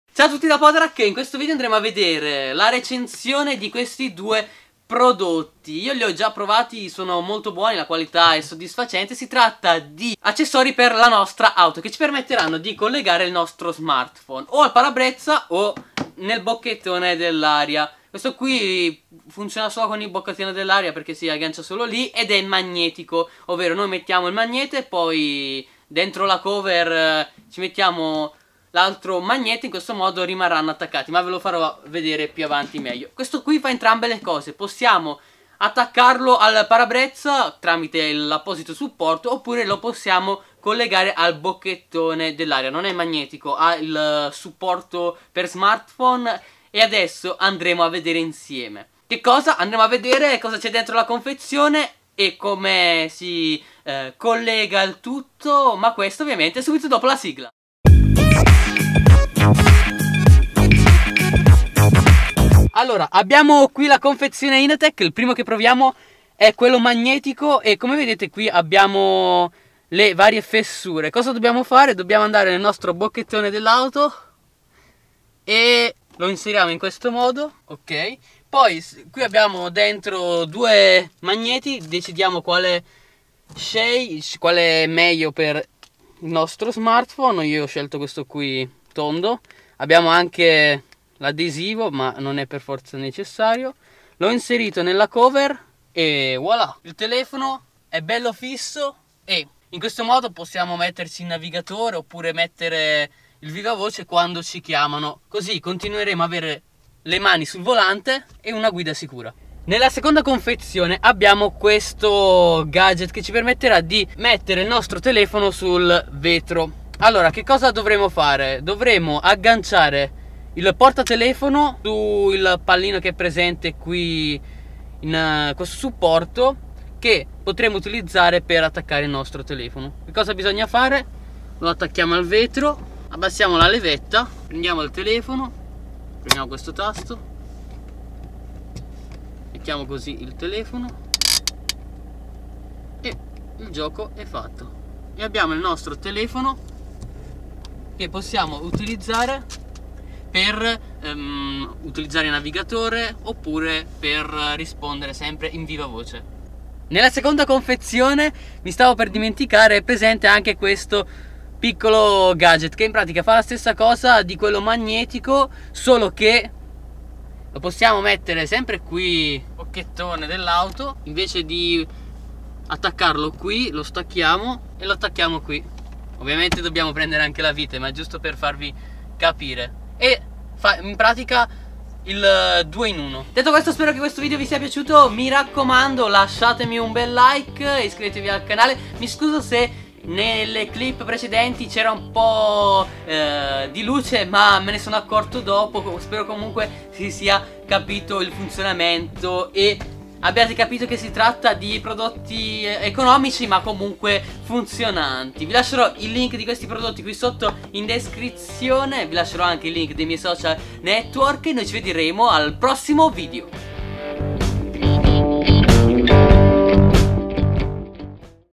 Recensione supporto smartphone per auto - Inateck
Dei semplici supporti smartphone per auto adatti per ogni esigenza.

- Supporto smartphone magnetico Inateck
- Supporto smartphone Inateck